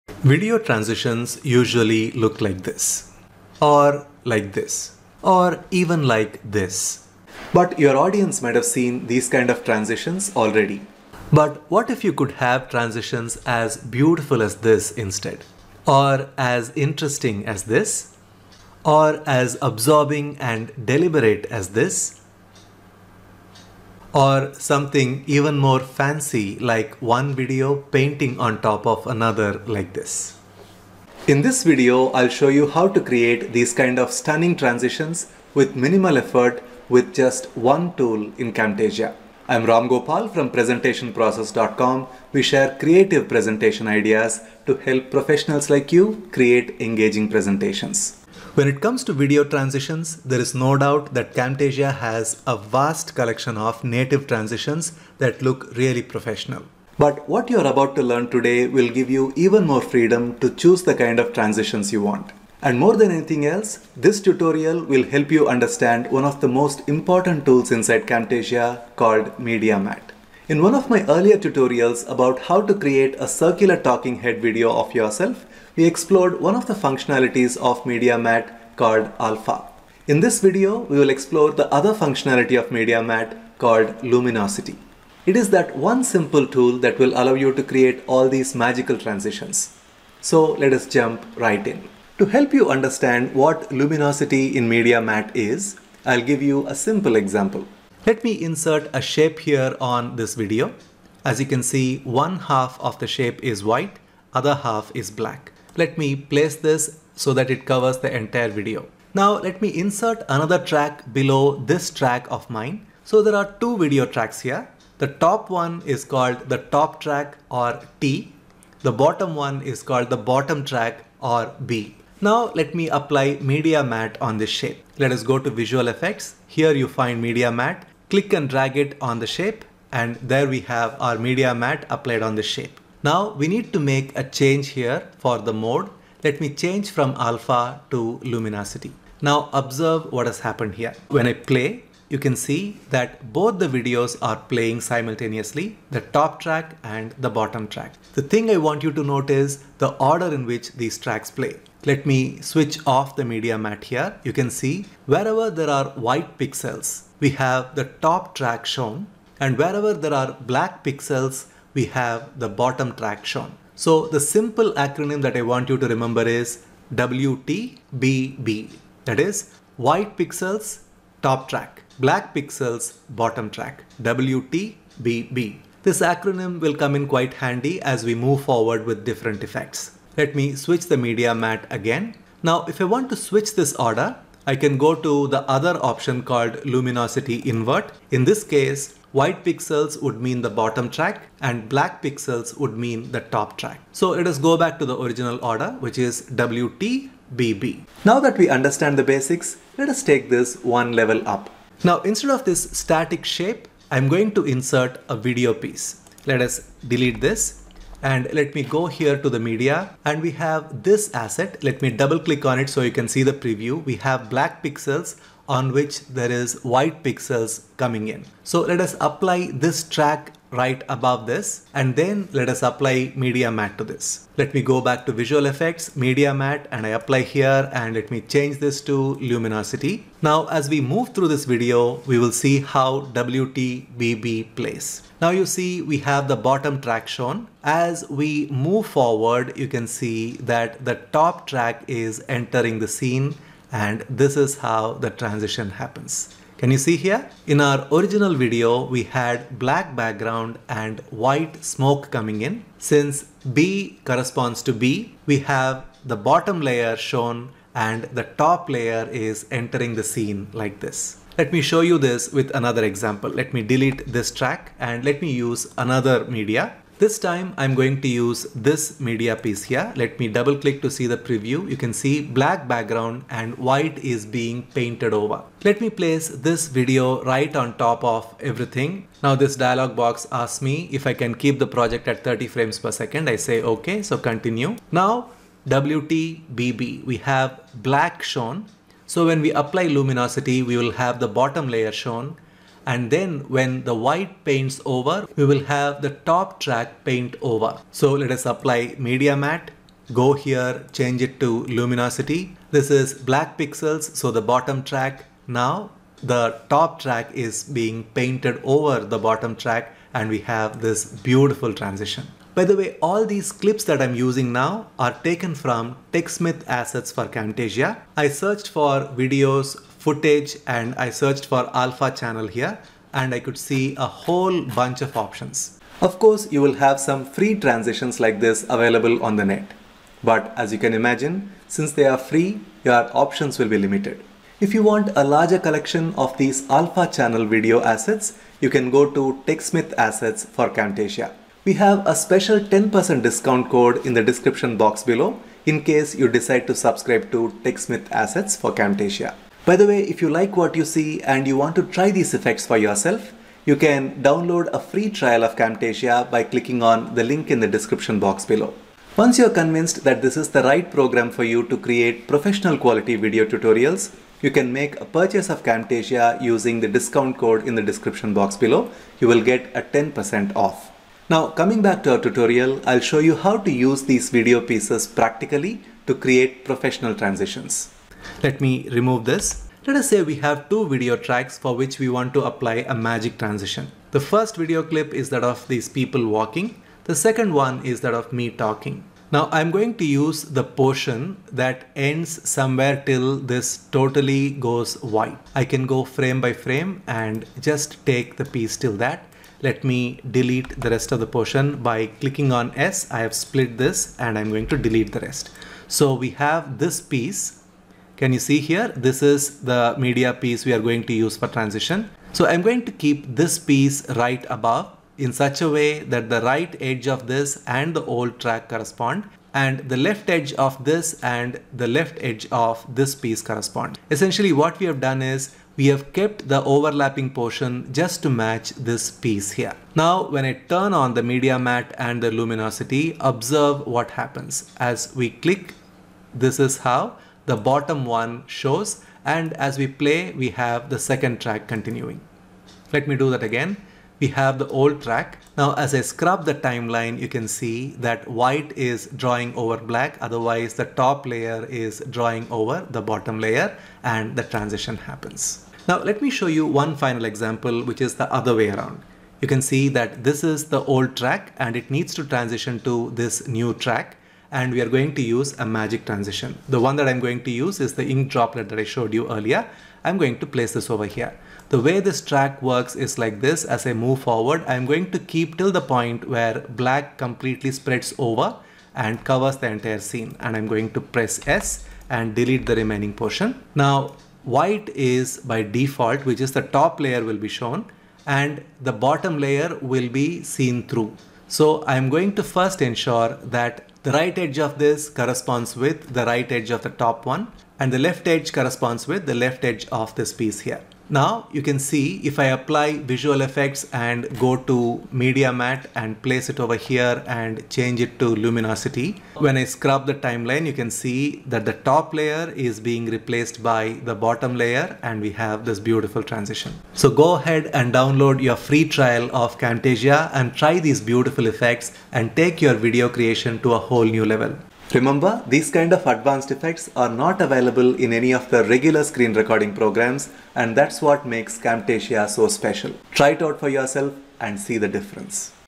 Stunning Video Transitions You Didn't Know About [Media Matte in Camtasia]
Discover how you can create stunning video transitions using Media Matte feature in Camtasia.

Many thanks to Techsmith, makers of Camtasia for sponsoring this video.

Contents of this video:
00:00 Preview of Transitions
00:48 What is Media Matte & Luminosity
03:18 Add Custom Transitions in Camtasia
05:52 Accessing Camtasia
06:17 Creating Professional Transitions